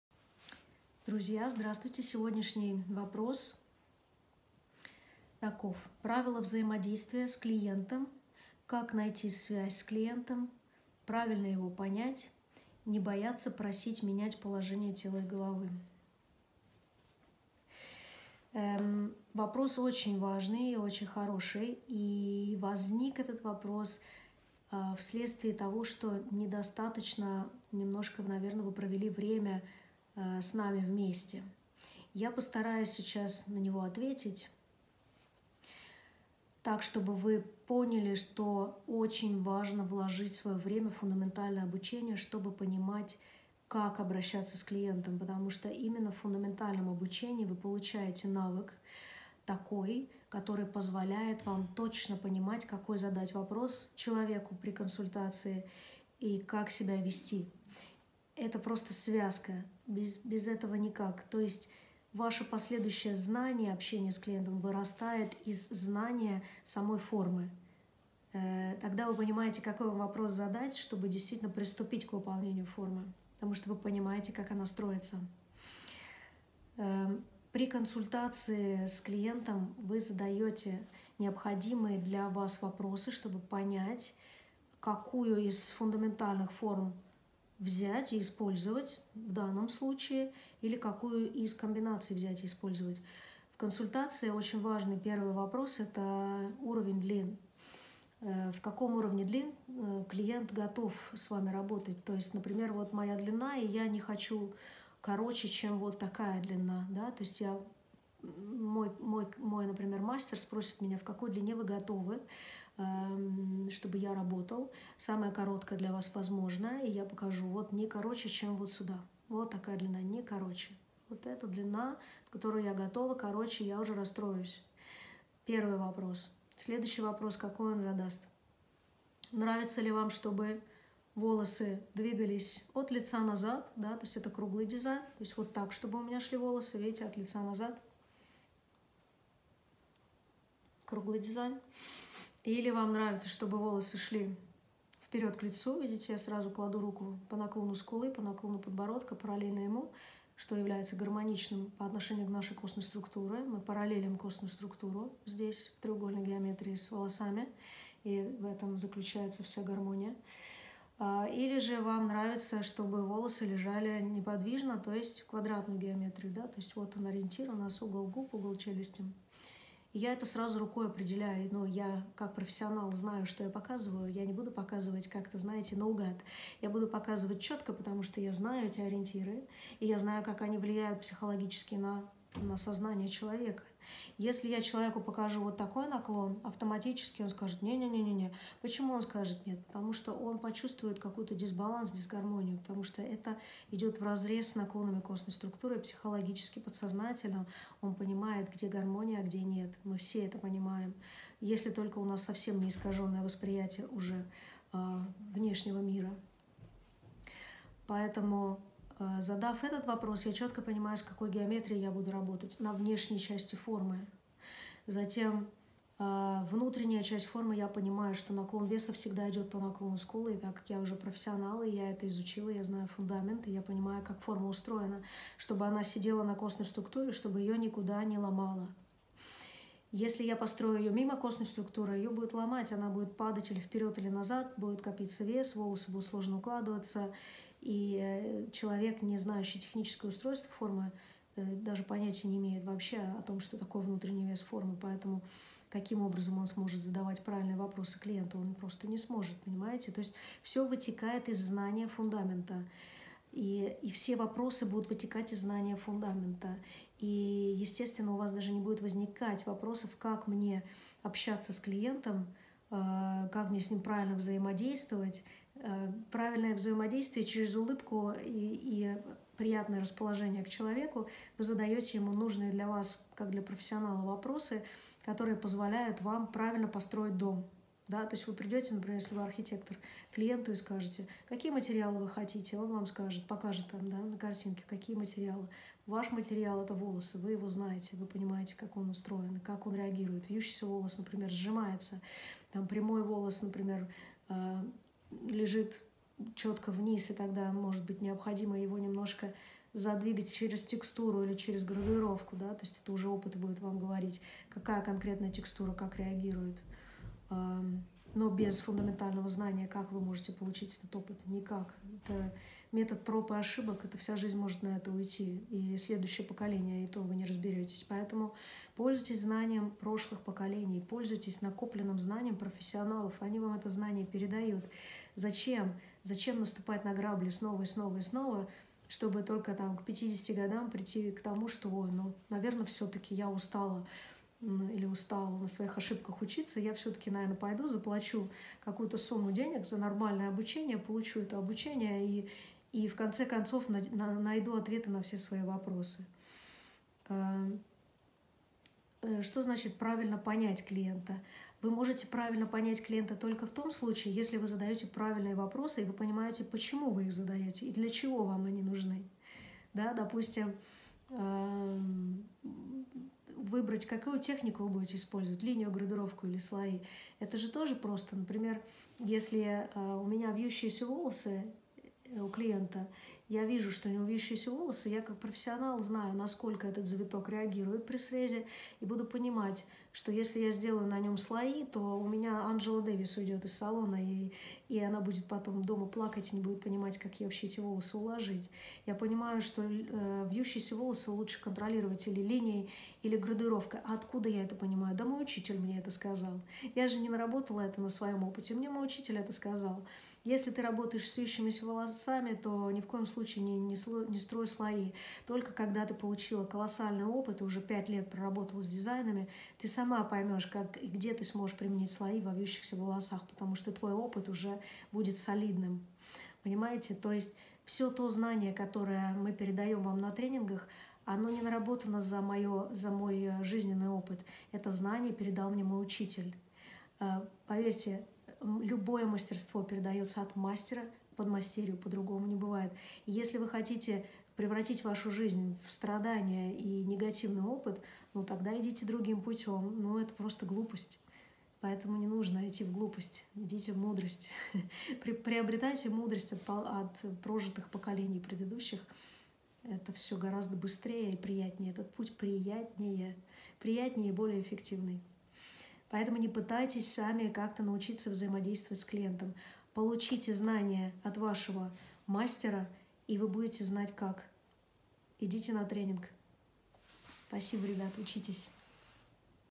ANNA ESHWOOD. Правила взаимодействия с клиентом
Как грамотно взаимодействовать с клиентами? Как найти подход к каждому?